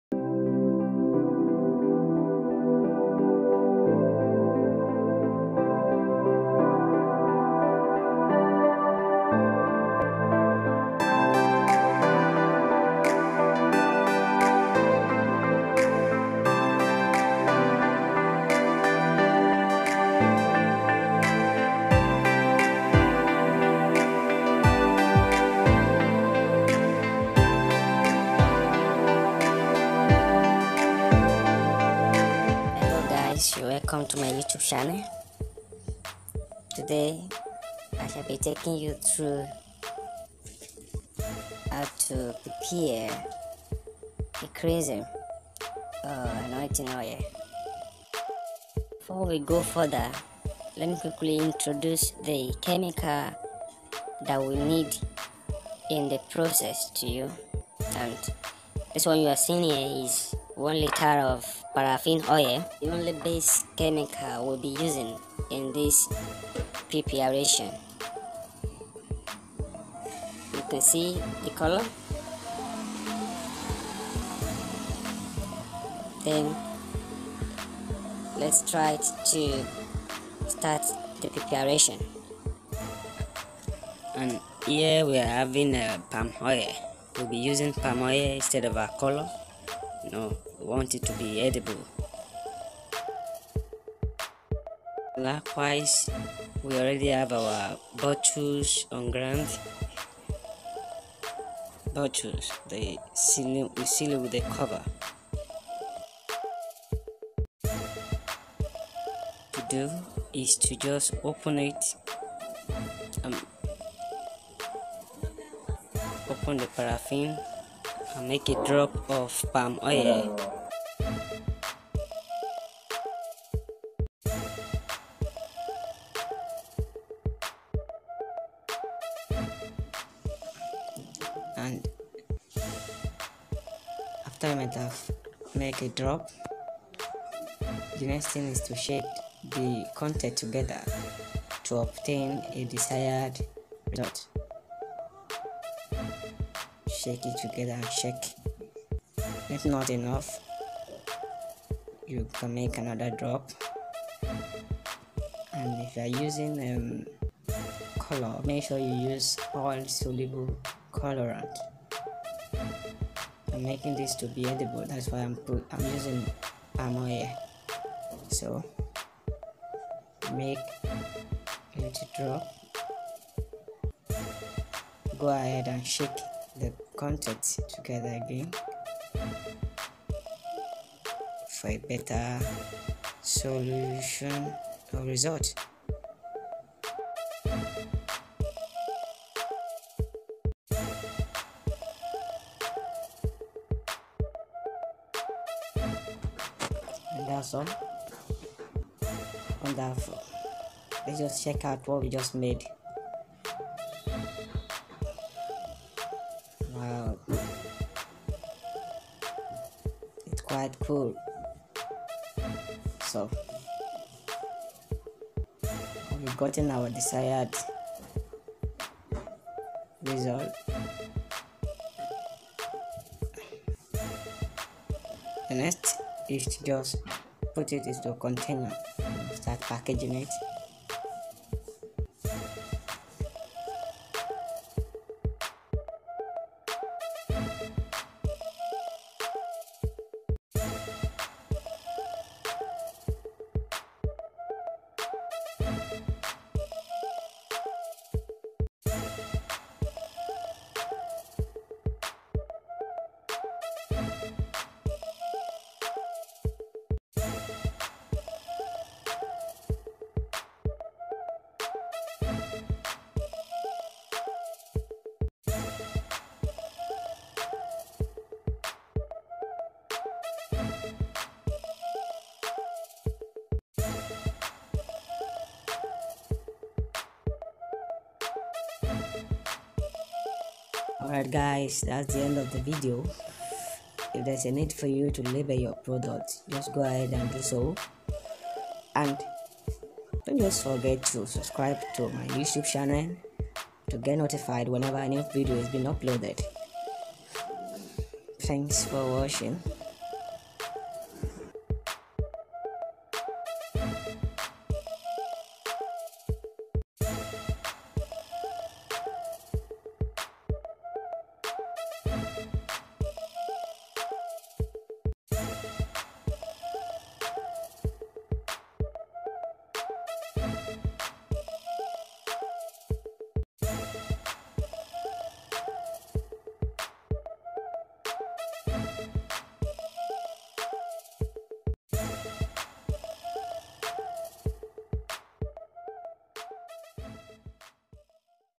How to prepare anointing oil or chrism used in ceremonies
Chemicals in quantity

1. The only active chemical is Parafin oil or you call it mineral oil which is obtainable from crude oil.

2. And for the colour, is either you use oil soluble colourant or You use Palm oil. In this video, we shall be making use of Palm oil.

3. Perfume_ unless You were being asked to make a perfume anointing, there is no essence of doing so I.e it's optional